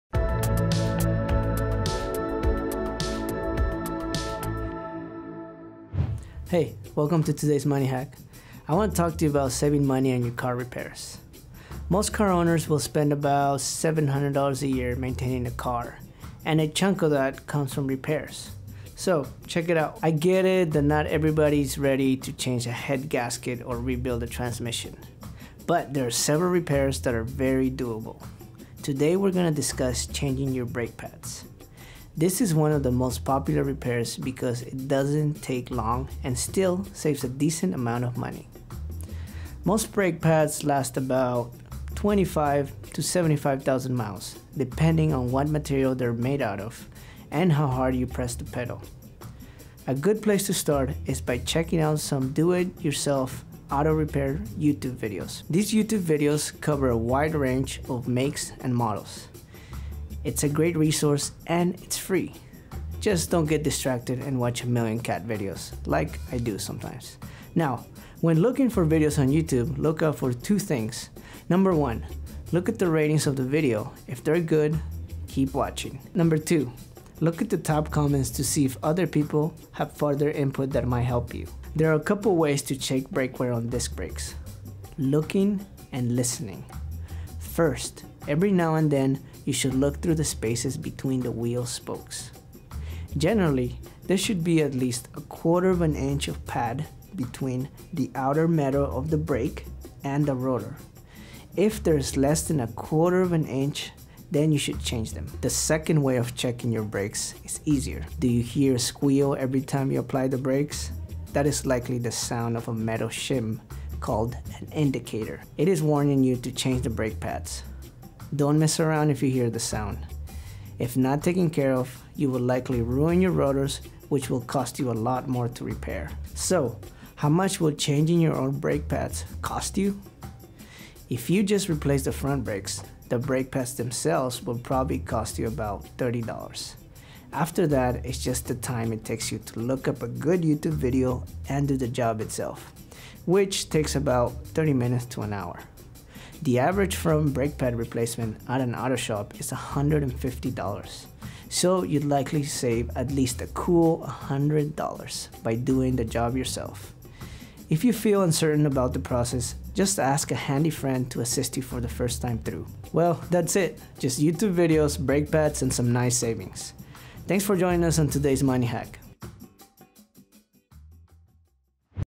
Yes! There are car repairs you can do yourself. - Stupid Money TV
Stewardship Ministries, North American Division of Seventh-day Adventists: MoneyHack - Yes! There are car repairs you can do yourself.
Watch this quick MoneyHack video to learn how doing your own car repair doesn't have to be a huge time sink and can save you a decent amount of money. Visit stupidmoneytv.com for more tips on how to save.